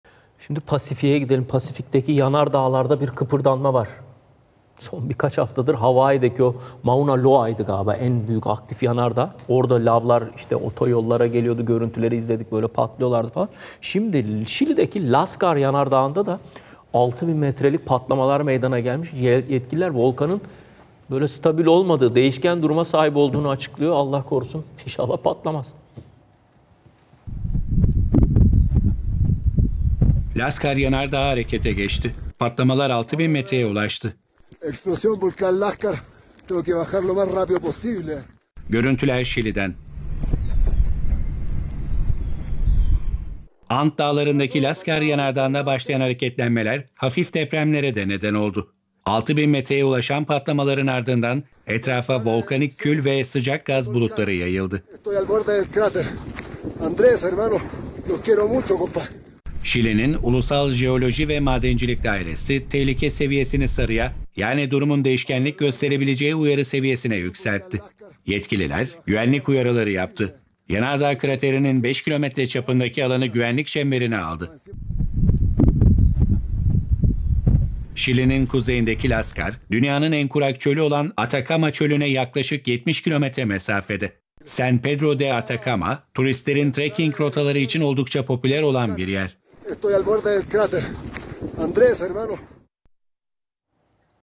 Lascar yanardağı harekete geçti!
Şili’deki Lascar yanardağında 6 bin metrelik patlamalar meydana geldi. Yetkililer, volkanın değişken duruma sahip olduğunu açıkladı.

And Dağları’ndaki Lascar yanardağında başlayan hareketlenmeler, hafif depremlere de neden oldu. 6 bin metreye ulaşan patlamaların ardından etrafa volkanik kül ve sıcak gaz bulutları yayıldı. Şili'nin Ulusal Jeoloji ve Madencilik Dairesi, tehlike seviyesini sarıya, yani durumun değişkenlik gösterebileceği uyarı seviyesine yükseltti. Yetkililer, güvenlik uyarıları yaptı. Yanardağ kraterinin 5 kilometre çapındaki alanı güvenlik çemberine aldı.

Şili'nin kuzeyindeki Lascar, dünyanın en kurak çölü olan Atacama Çölü’ne yaklaşık 70 km mesafede. San Pedro De Atacama turistlerin trekking rotaları için oldukça popüler olan bir yer.